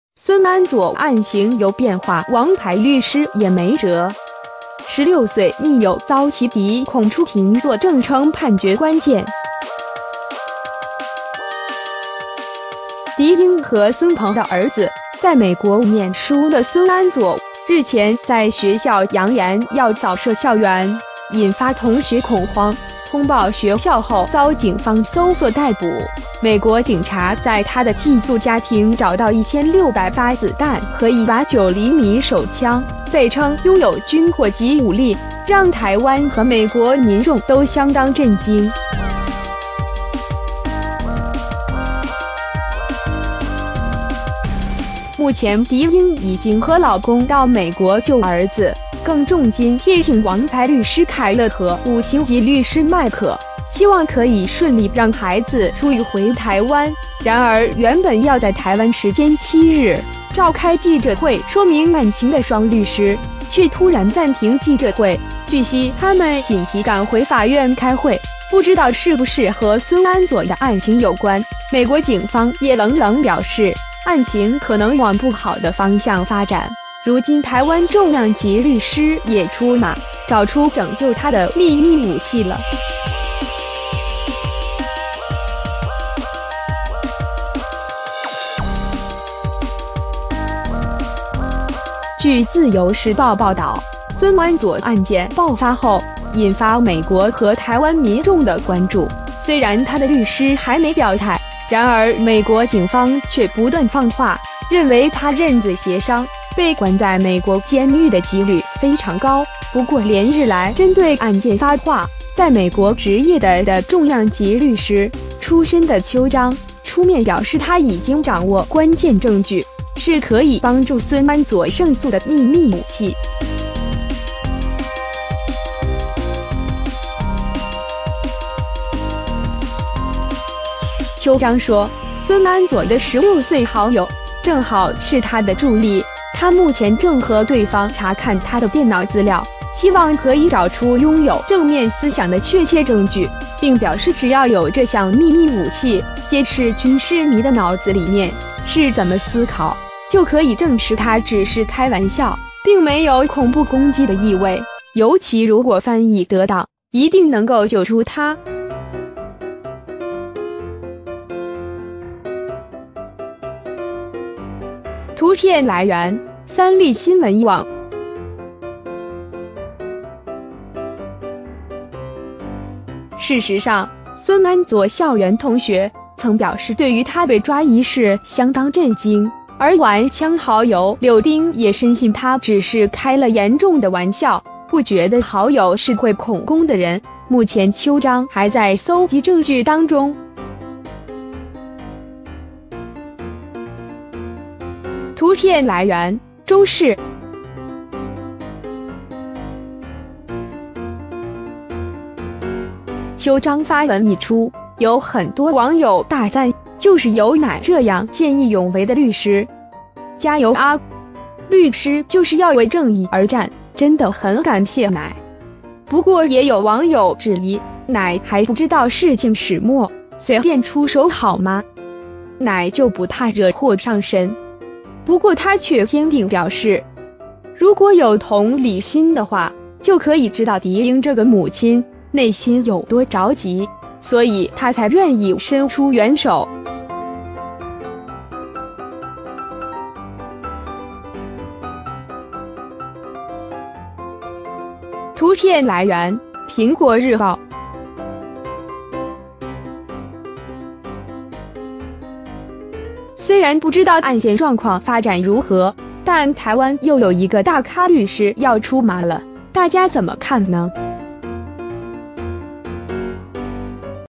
孙安佐“案情有变化”王牌律师也没辙？！16岁密友遭起底“恐出庭作证”成判决关键！
狄莺和孙鹏的儿子，在美国念书的孙安佐，日前在学校扬言“要扫射校园”，引发同学恐慌，通报学校后遭警方搜索逮捕，美国警察在他的寄宿家庭找到1600发子弹和一把9厘米手枪，被称拥有“军火级武力”，让台湾和美国民众都相当震惊！

目前狄莺已经和老公到美国救儿子，更重金聘请王牌律师凯勒和“五星级律师”麦可，希望可以顺利让孩子出狱回台湾，然而原本要在台湾时间7日，召开记者会说明案情的双律师，却突然暂停记者会，据悉他们紧急赶回法院开会，不知道是不是和孙安佐的案情有关，美国警方也冷冷表示，案情可能往不好的方向发展，如今台湾重量级律师也出马，找出拯救他的“秘密武器”了！

据《自由时报》报导，孙安佐案件爆发后，引发美国和台湾民众的关注，虽然他的律师还没表态，然而美国警方却不断放话，认为他认罪协商，被关在美国监狱的机率非常高；不过连日来针对案件发话，在美国执业的的重量级律师，出身的邱彰，出面表示她已经掌握“关键证据”，是可以帮助孙安佐胜诉的秘密武器！

邱彰说，孙安佐的16岁好友，正好是她的助理，她目前正和对方查看他的电脑资料，希望可以找出拥有“正面思想”的确切证据，并表示只要有这项祕